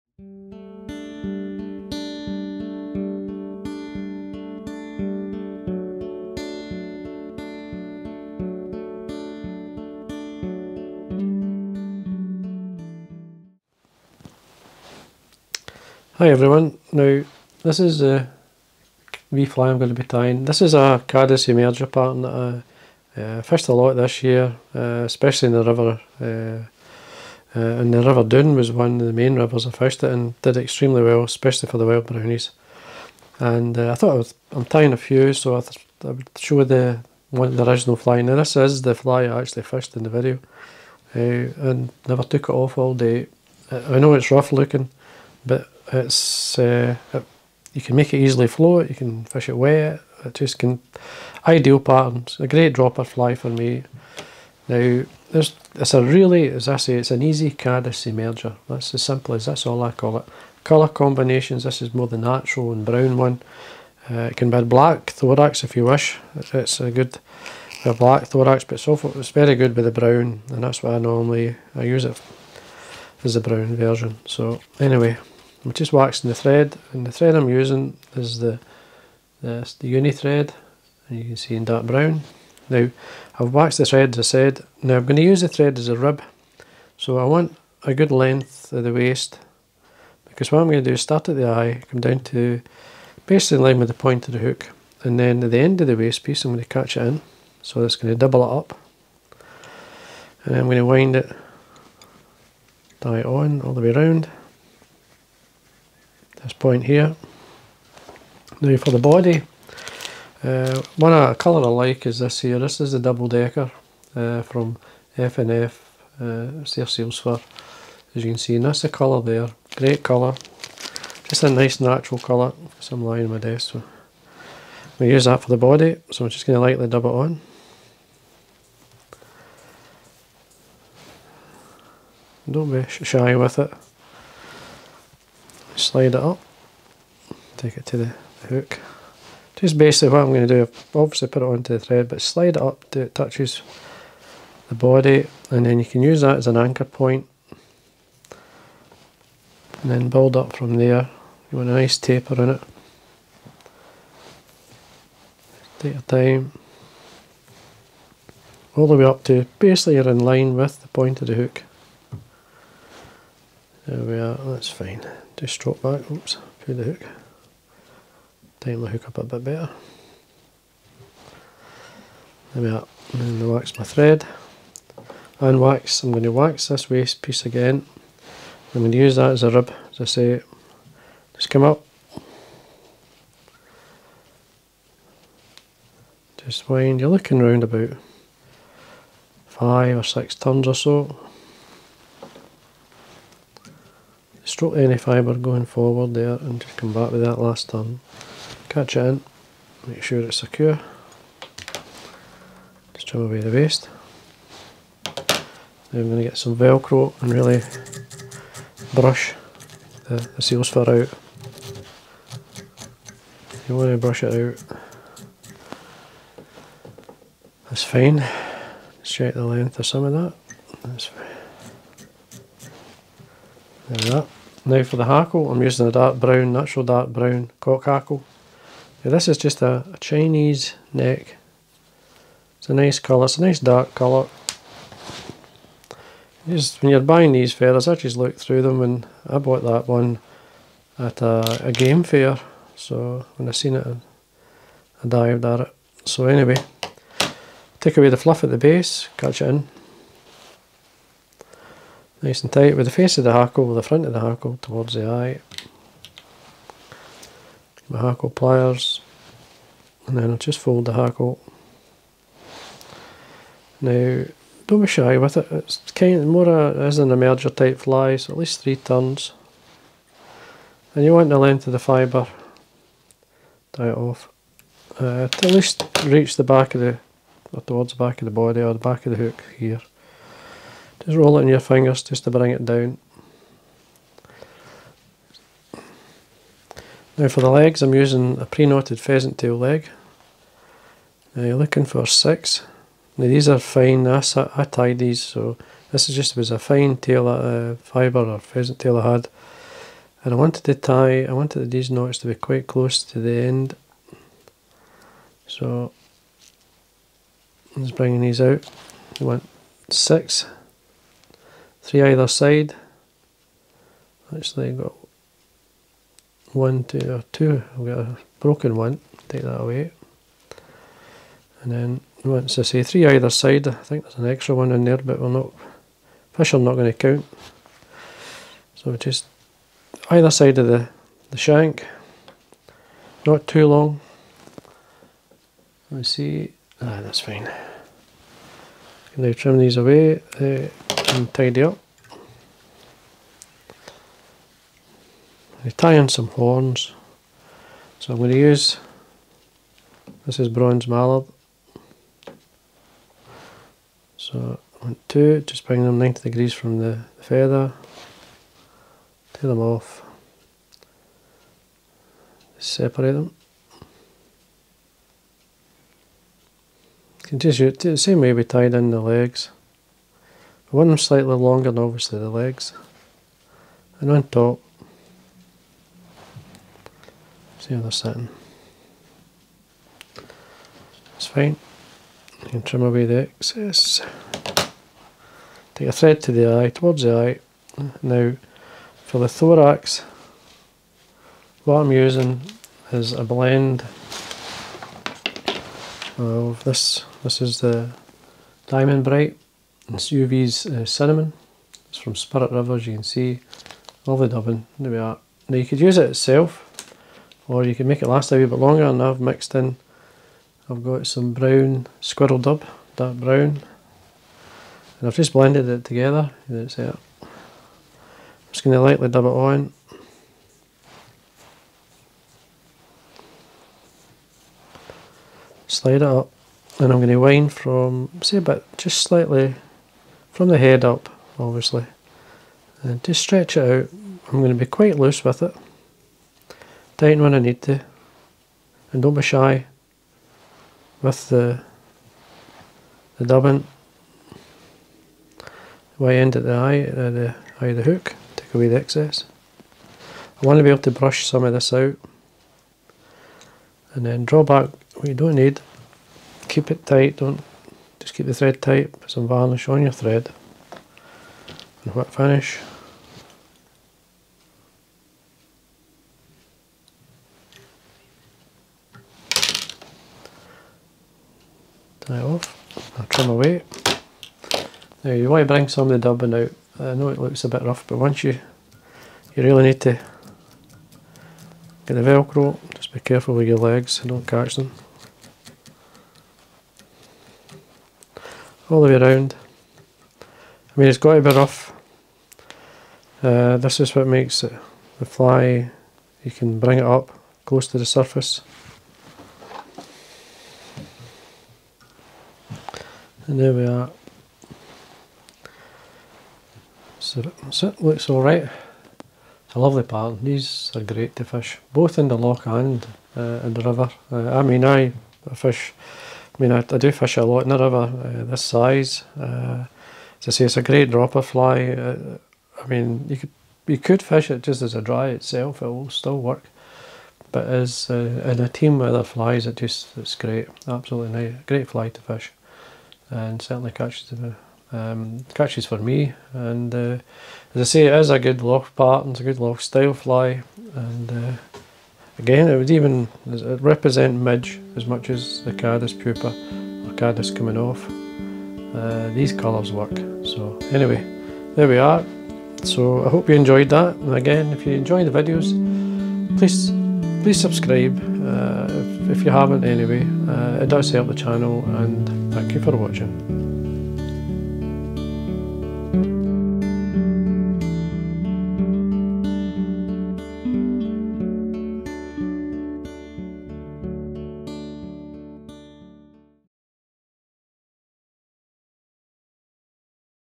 Tying an Easy Leggy Caddis Fly Emerger by Davie McPhail
Hook, Fulling Mill Czech Nymph Size 16
Thread, Dark Brown 8/0 Uni-Thread
Rib, Double Strand of the Dark Brown Uni-8/0
Body, FNF Double Decker Seals Fur
Legs, 6 Pre-Knotted Pheasant Tail Fibres
Hackle, Natural Brown Cock
Horns, Bronze Mallard Fibres
Thorax, Dark Brown Squirrel Dubbing and UV Diamond Brite mixed together..